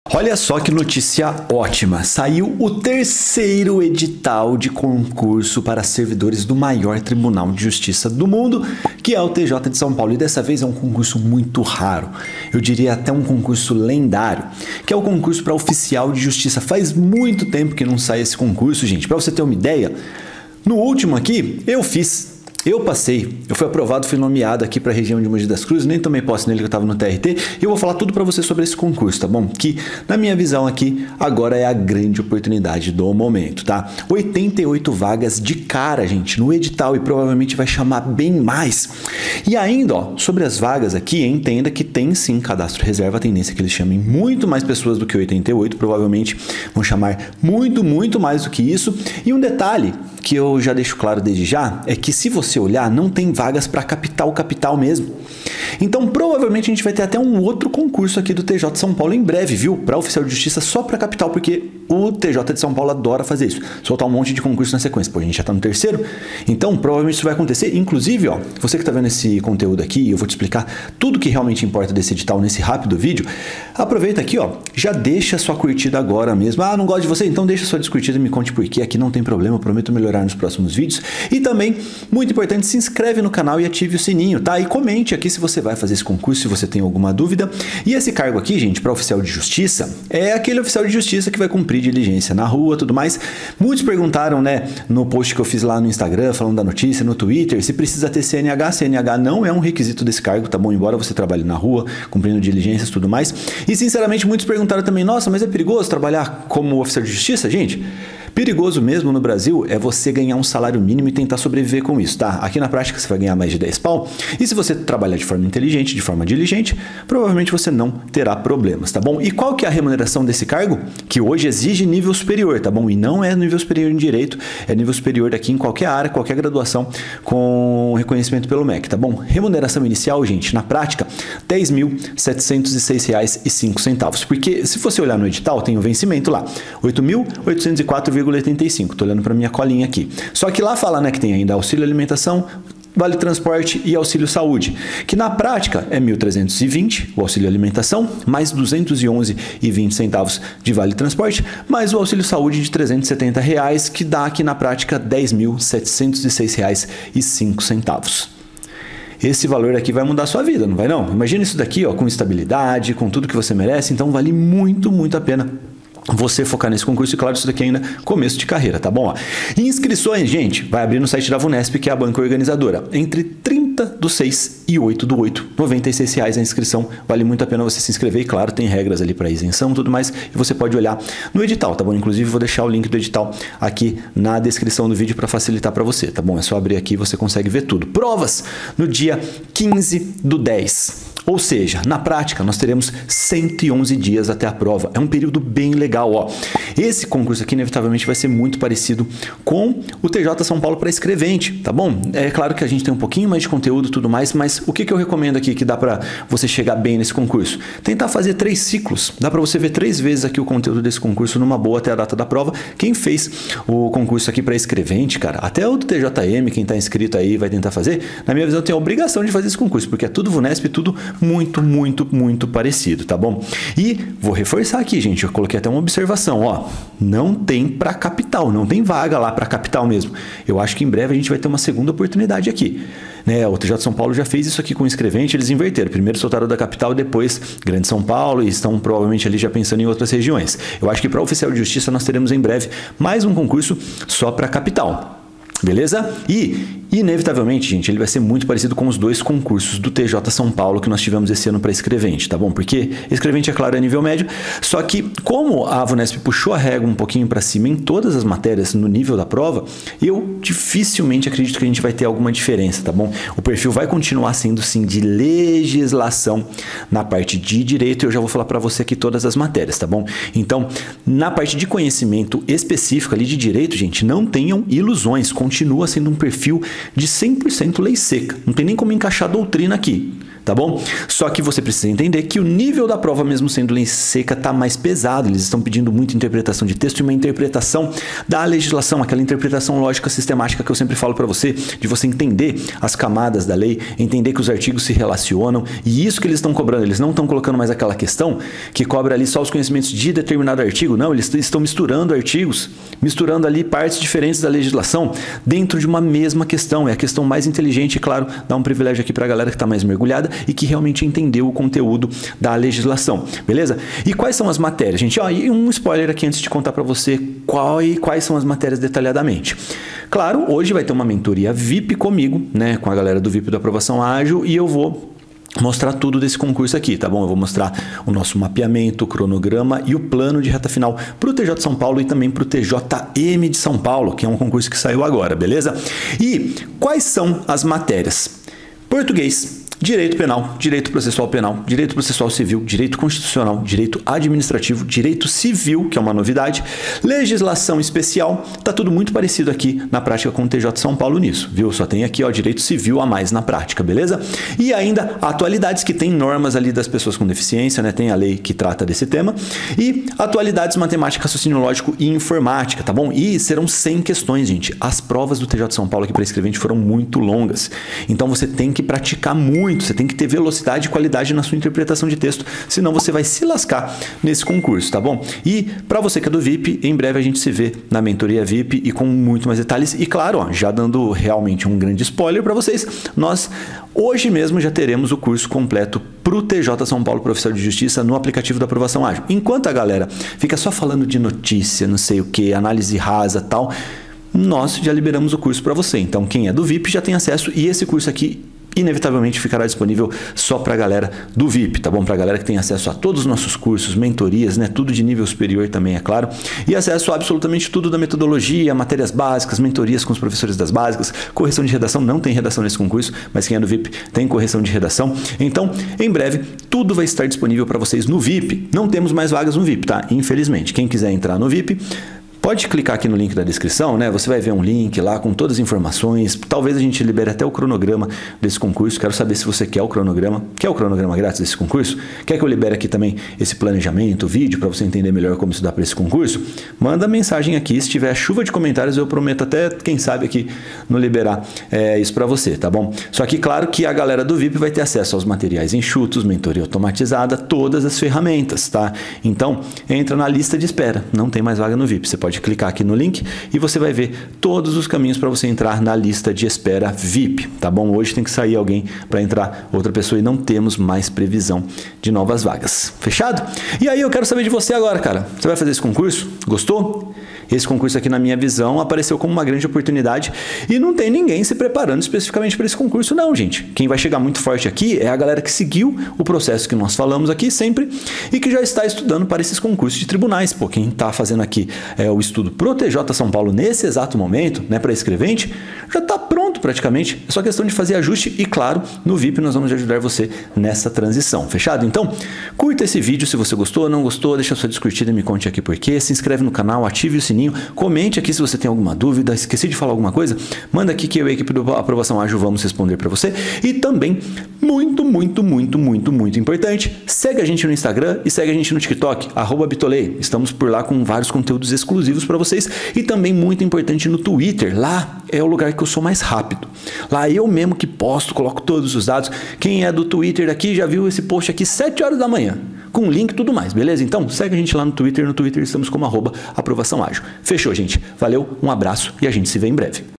[URGENTE!] Saiu mais um CONCURSO PÚBLICO do TJ SP em 2023 (não é para ESCREVENTE)
👉🏼Nesse vídeo você vai descobrir tudo sobre o CONCURSO PÚBLICO para Oficial de Justiça do TJ SP em 2023 (não é para ESCREVENTE)

🎙️Você pode ver o edital do concurso público TJ SP para oficial de justiça 2023 no site da VUNESP

📚Quer baixar grátis meus cronogramas e participar de eventos exclusivos? Entre no link agora